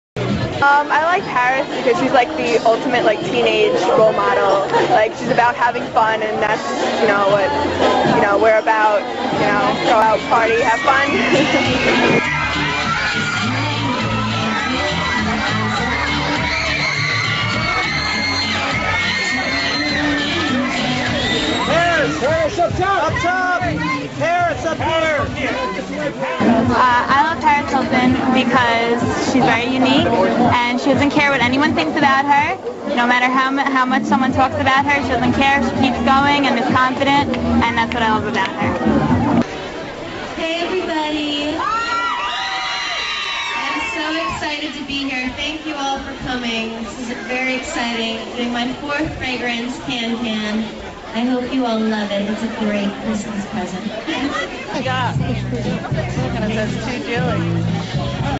Paris Hilton Visita "Long Island"
Paris Hilto , visita Long Island para promocionar su Perfume CAN CAN , que esta marcando altas ventas en donde se presenta para promocionarlo.